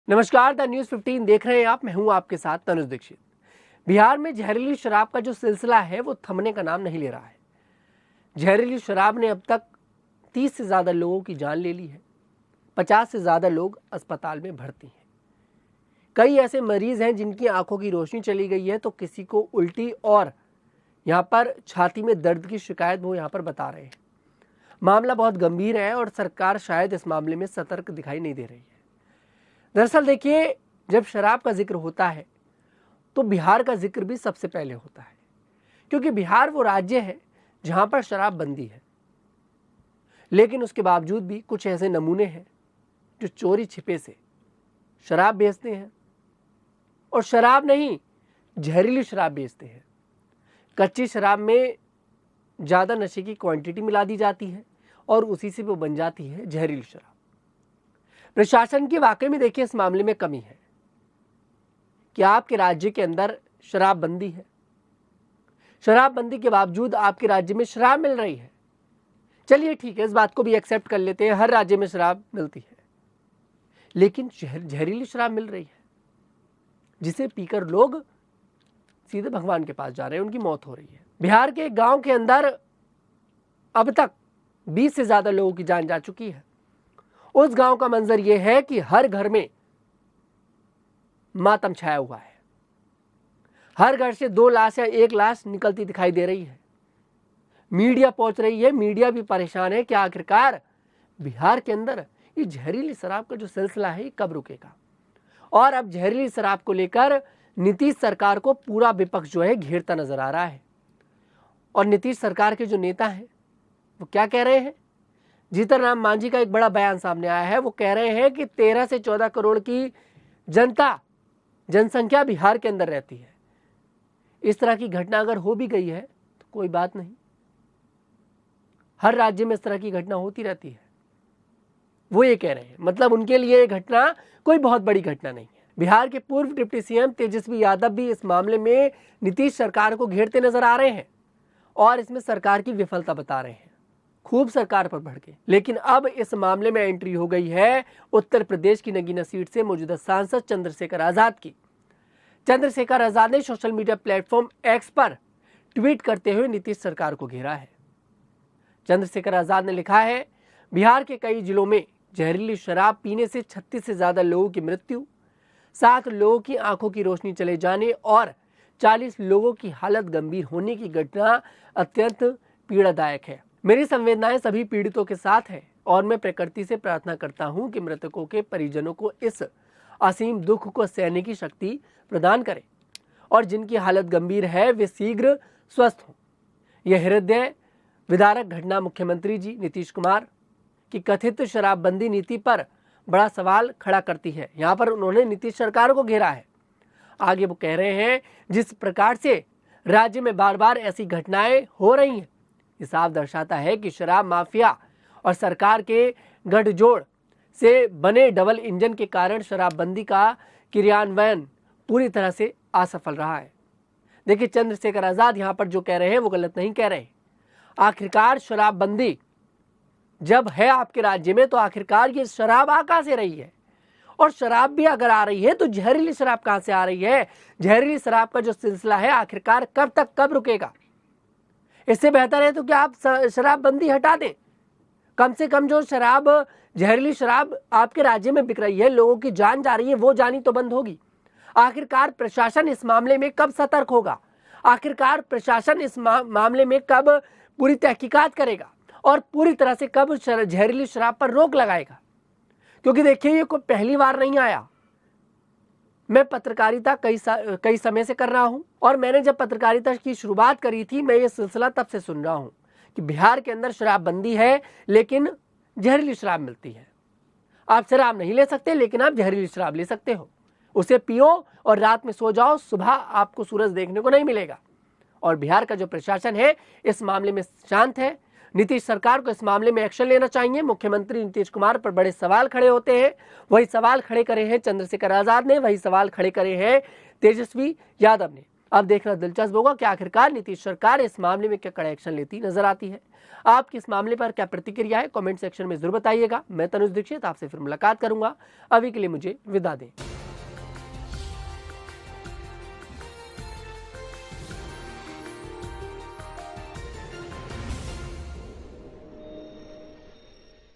Bihar News: जहरीली शराब ने Bihar में उजाड़े कई घर, Chandrashekhar ने सरकार को खूब सुनाई |Nitish Kumar
Bihar Live: जहरीली शराब ने Bihar में उजाड़े कई घर, Chandrashekhar ने सरकार को खूब सुनाई | Nitish Kumar | The News15